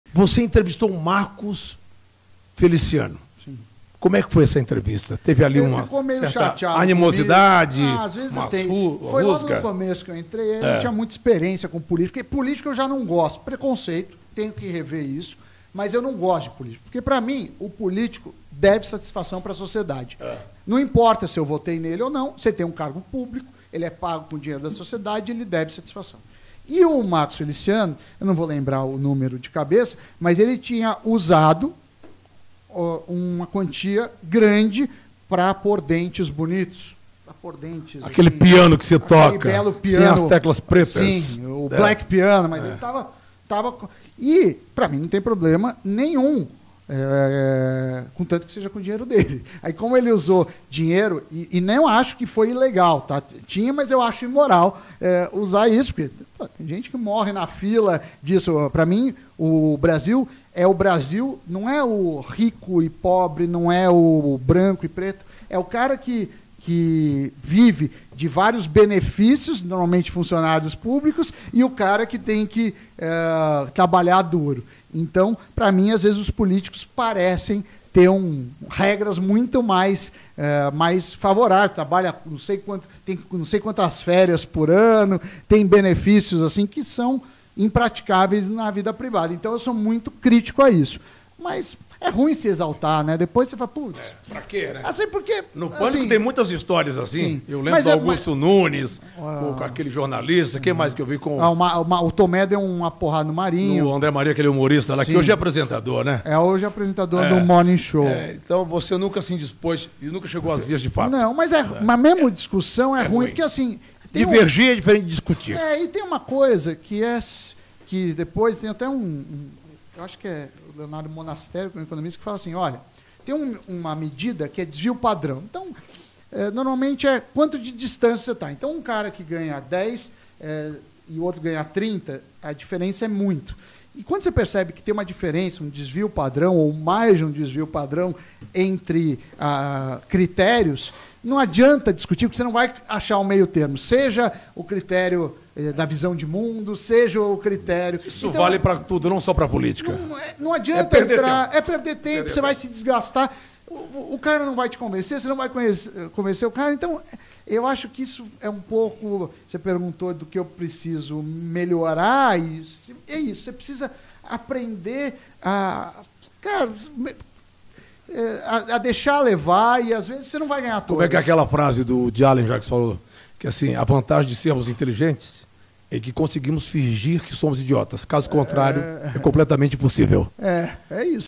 PR3CONCEIT0 COM POLÍTICO??
Vem ver o Samy Dana contando sobre o dia que entrevistou o Marco Feliciano! Rolou briga??

Festa da Firma Podcast está disponível no YouTube e nas plataformas de áudio Spotify, Deezer e Apple Podcasts.